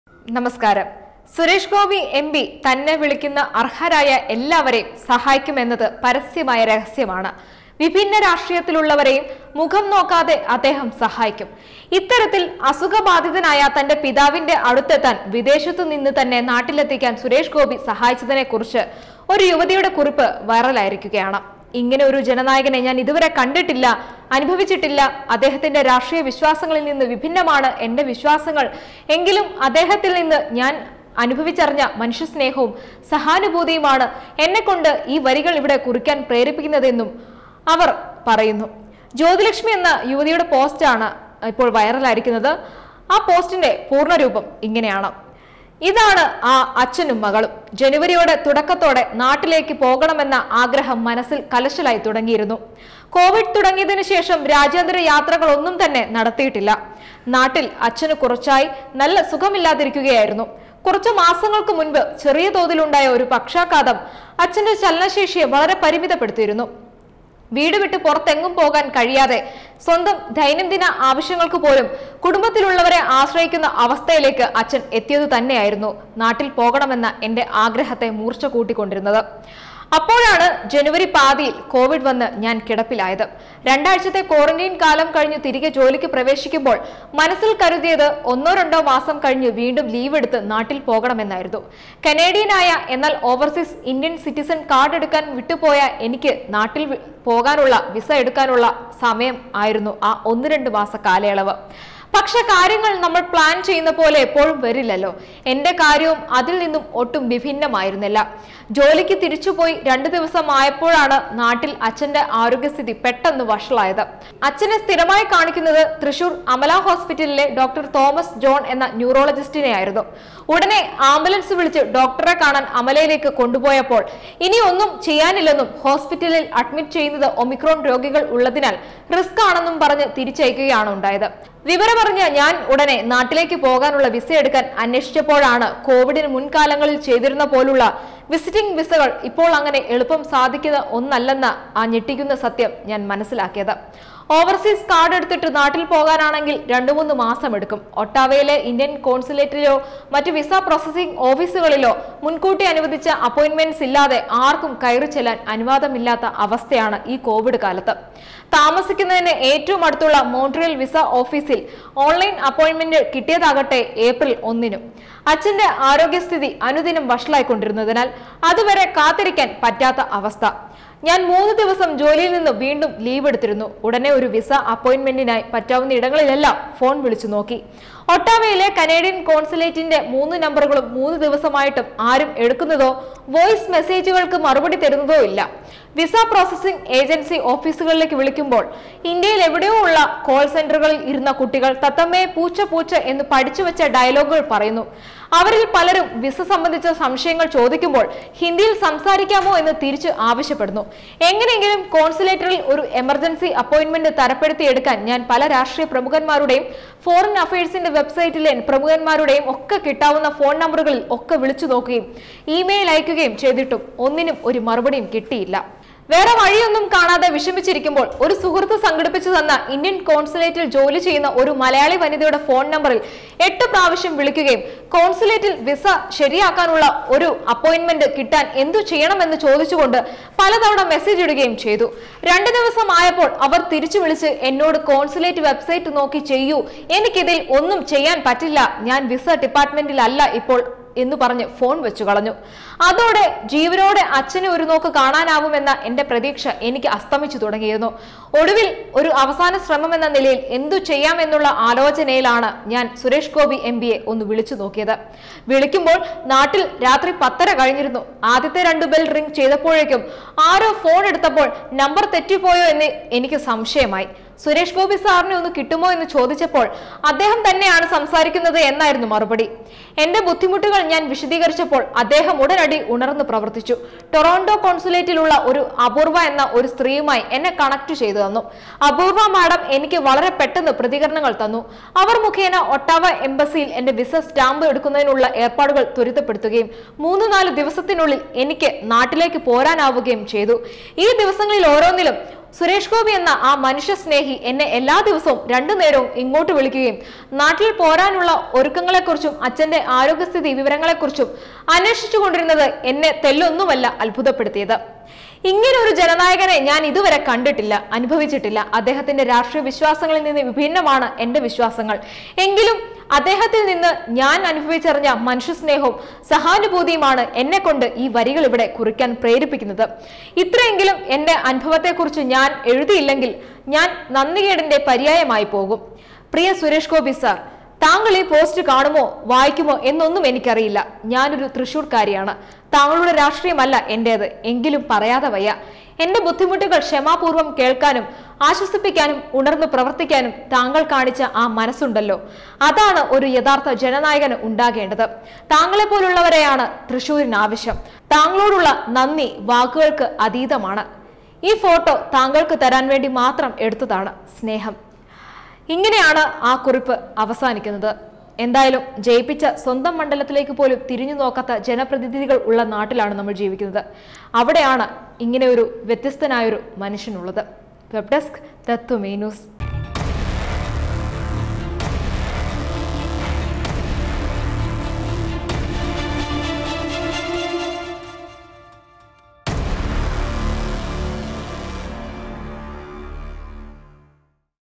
'താങ്കളേപ്പോലുള്ളവരെ ആണ് തൃശൂരിന് ആവശ്യം'; സുരേഷ്‌ഗോപിയെ പറ്റി ഹൃദയ സ്പർശിയായ കുറിപ്പുമായി ഒരു യുവതി
താങ്കളേപ്പോലുള്ളവരെ ആണ് തൃശൂരിന് ആവശ്യം; സുരേഷ്‌ഗോപിയെ പറ്റി ഹൃദയ സ്പർശിയായ അനുഭവം പങ്കുവെച്ച് പ്രവാസി യുവതി | SURESHGOPI